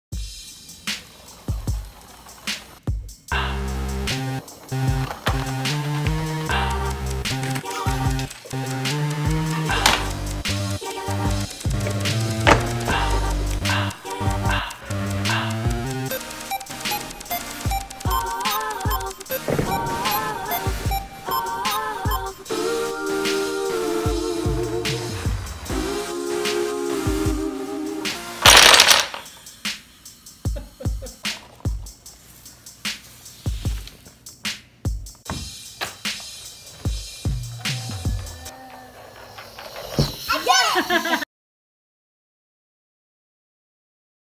Kazoo magazine Balloon-Powered Car Stunts
Find step-by-step instructions on how to make a balloon-powered car out of a juice box in the latest issue of Kazoo magazine (#08 The Mistakes Issue). We designed ours w the help of Toyota Global engineer Jackie Birdsall. Then we had some fun!!! (Tiny Editor helped us lay down beats on Garage Band. Turn up the volume for full effect.)